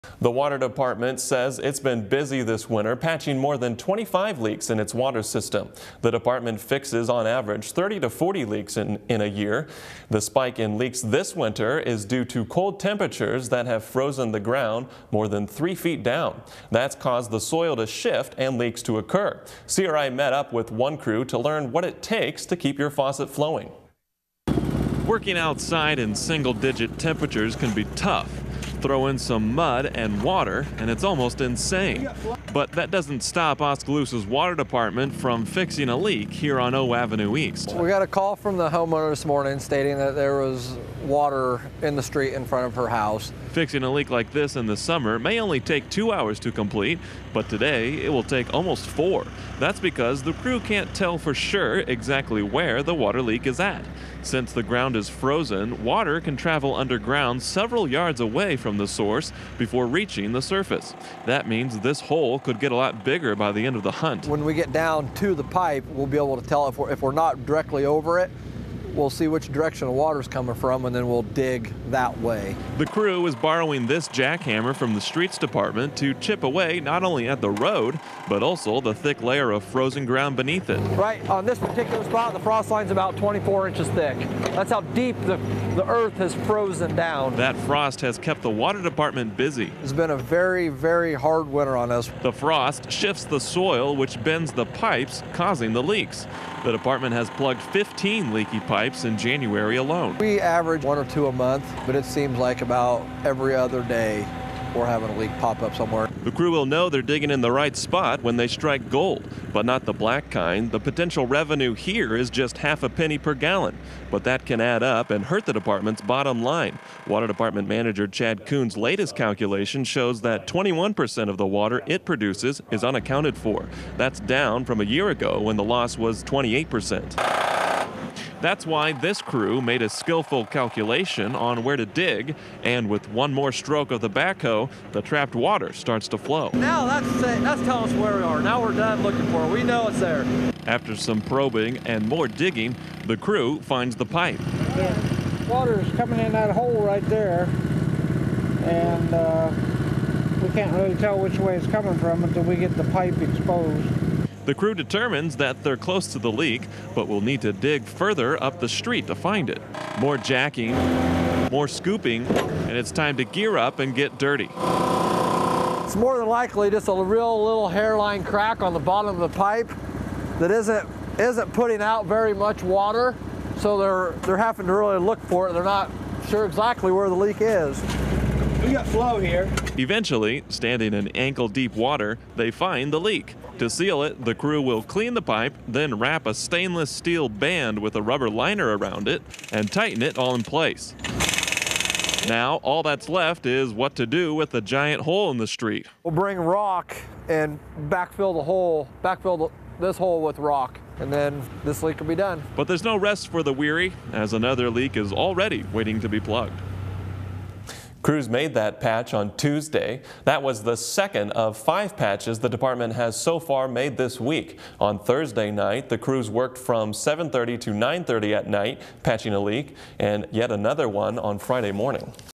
Cold Causes Leaky Water Mains
The water department says its been busy this winter patching more than 25 leaks in it's water system. The department fixes on average 30-40 leaks in year. The spike in leaks this winter is due to cold temperatures that have frozen the ground more than 3 feet down. That's caused the soil to shift and leaks to occur. CRI met up with one crew to learn what it takes to keep your faucet flowing.